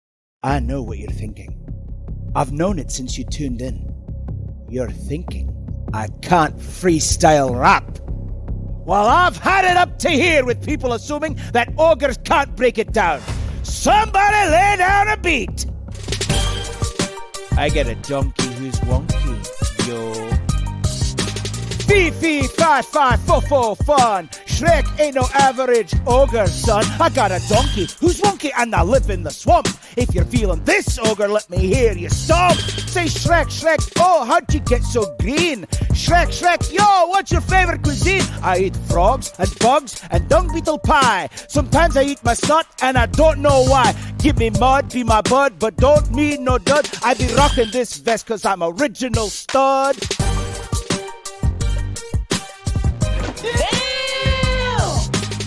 Shrek Drops the Mic with his perfomance
In this Shrek Free Style Rap, we've gathered some of the most talented wordsmiths from the far, far away kingdom to drop verses that would make Donkey proud. 🎤🐴

Join us as we unleash the ogre-sized rhymes, clever references, and hilarious bars that pay tribute to the greenest hero of them all. From Shrek's onion-loving tendencies to his epic adventures with Fiona, no aspect of this beloved franchise is safe from our lyrical prowess.

Whether you're a lifelong Shrek fan or just here for some rhyming fun, our rap battles and verses will have you nodding in agreement and laughing out loud. Plus, we might even throw in a few surprise appearances from your favorite fairy tale characters.